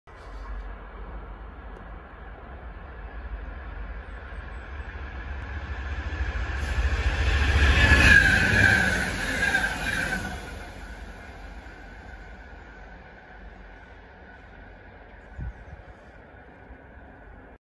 GBRf 66738, 66746 & 66745 Passes Aycliffe | 0S15 Doncaster Down Decoy to Millerhill S.S.
66738 - ‘Huddersfield Town’
66746
66745 - ‘Modern Railways - The First 50 Years’